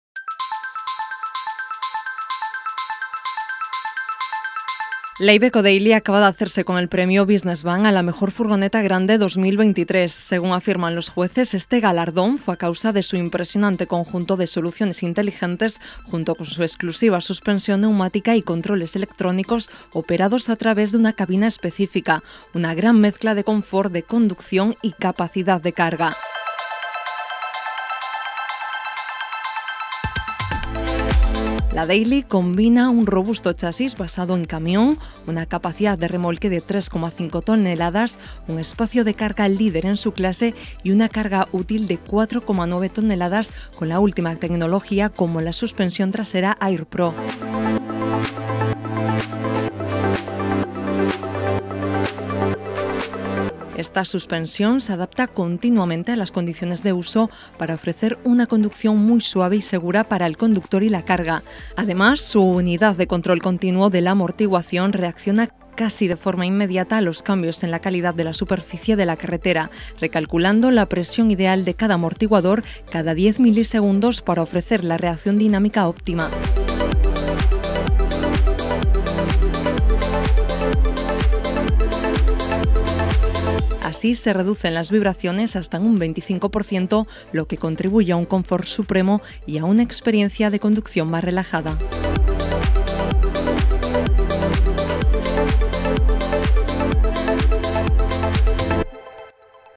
IVECO DAILY, LA “MEJOR FURGONETA GRANDE” | By CdRas
La Iveco Daily acaba de hacerse con el premio Business Van a la “Mejor furgoneta grande” 2023. Según afirman los jueces, este galardón fue a causa de su “impresionante conjunto de soluciones inteligentes junto con su exclusiva suspensión neumática y controles electrónicos operados a través de una cabina específica. Una gran mezcla de confort de conducción y capacidad de carga”.

La Daily combina un robusto chasis basado en camión, una capacidad de remolque de 3,5 toneladas, un espacio de carga líder en su clase y una carga útil de 4,9 toneladas con la última tecnología, como la suspensión trasera AIR-PRO.

Esta suspensión se adapta continuamente a las condiciones de uso para ofrecer una conducción muy suave y segura para el conductor y la carga. Además, su unidad de control continuo de la amortiguación reacciona casi de forma inmediata a los cambios en la calidad de la superficie de la carretera, recalculando la presión ideal de cada amortiguador cada 10 milisegundos para ofrecer la reacción dinámica óptima.

Así, se reducen las vibraciones hasta en un 25 %, lo que contribuye a un confort supremo y a una experiencia de conducción más relajada.